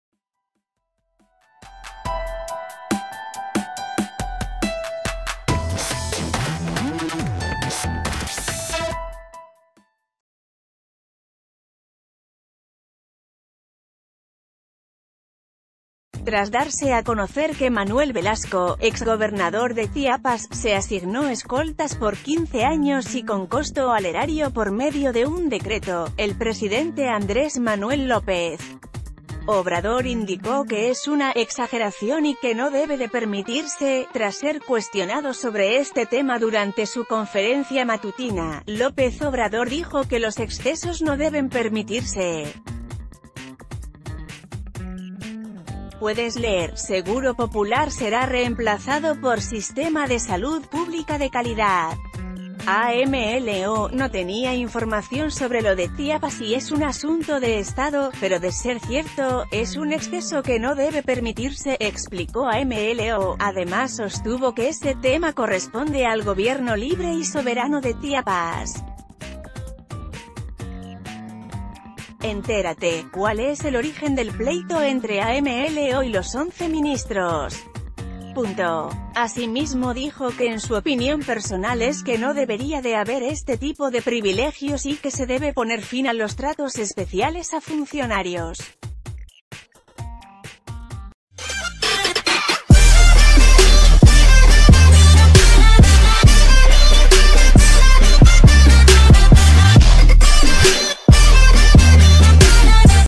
Una exageración que Manuel Velasco tenga escolta por 15 años: AMLO
Tras darse a conocer que Manuel Velasco, exgobernador de Chiapas, se asignó escoltas por 15 años y con costo al erario por medio de un decreto, el presidente Andrés Manuel López Obrador indicó que es una "exageración y que no debe de permitirse".Tras ser cuestionado sobre este tema durante su confer...